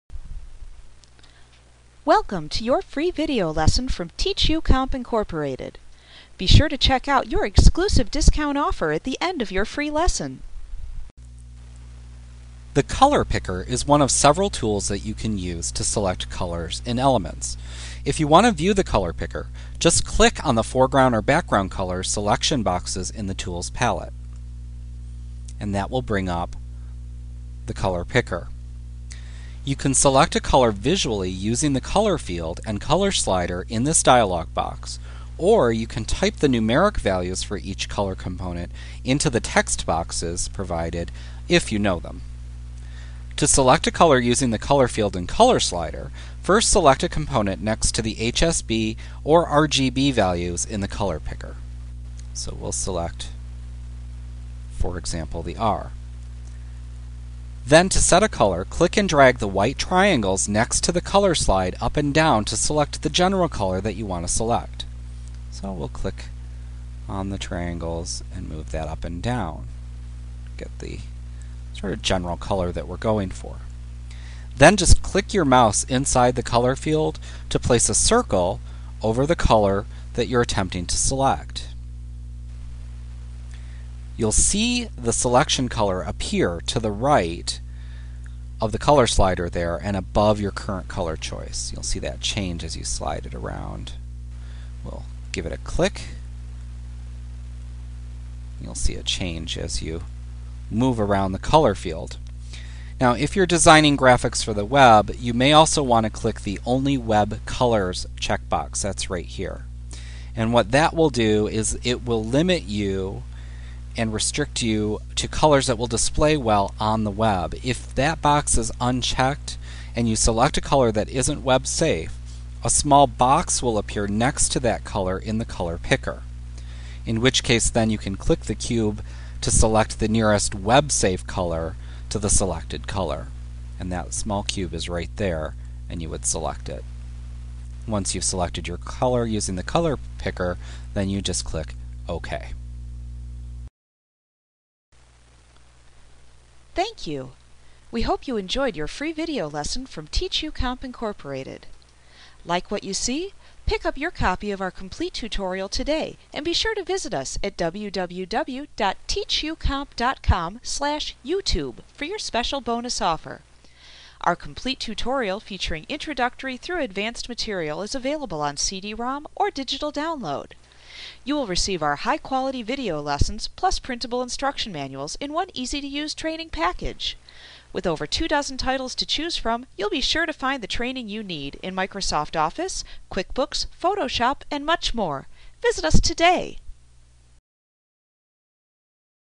Photoshop Elements Tutorial Using the Color Picker Adobe Training Lesson 5.5
Learn how to use the color picker in Adobe Photoshop Elements A clip from Mastering Photoshop Elements Made Easy v. 7. - the most comprehensive Photoshop Elements tutorial available. Visit us today!